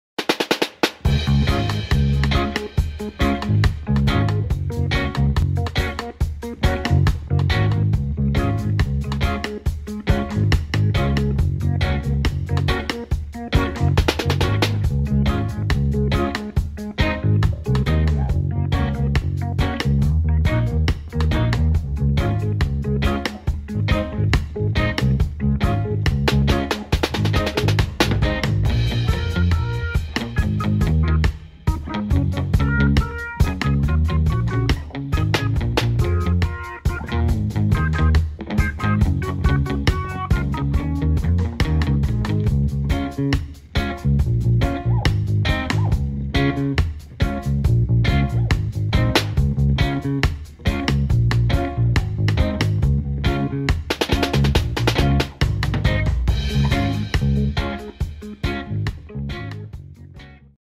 Handprint crab craft for preschoolers
Handprint crab craft for preschoolers by Yaswitha

Things needed-
Handprint paint or water colors
Poster colors for background
Googly eyes
Cotton for clouds
Glue
Links to different activities available in our channel: